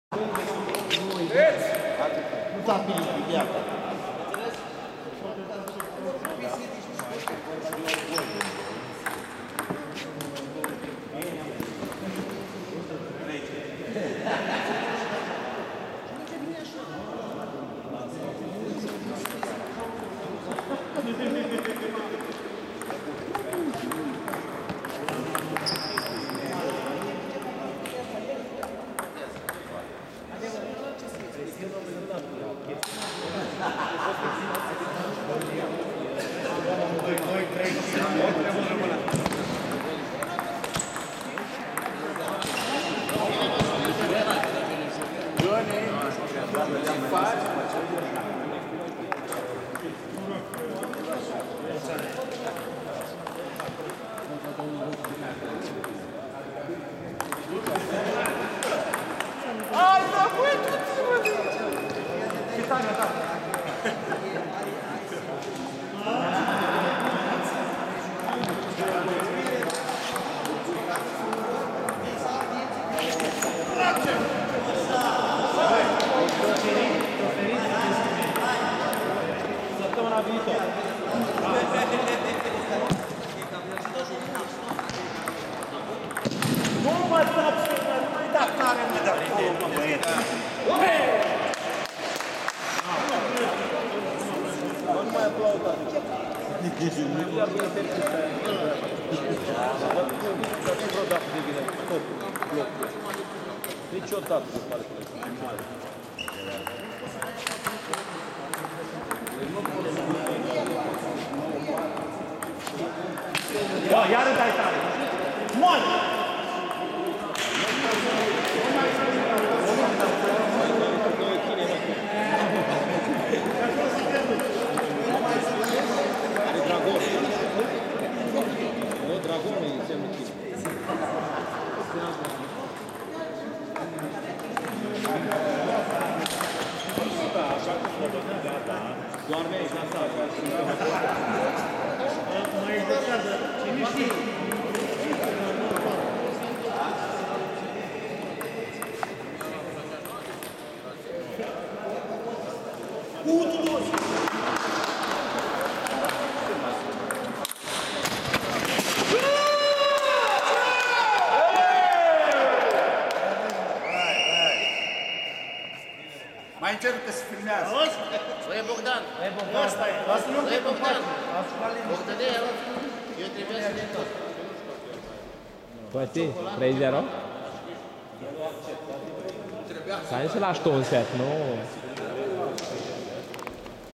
Corcoz Costin/ Ivanov Sorin si Capusa Bogdan / Marola Mihai(semifin.open) part. 2
Turneu B "Cupa Stejarul din Borzești", Onești - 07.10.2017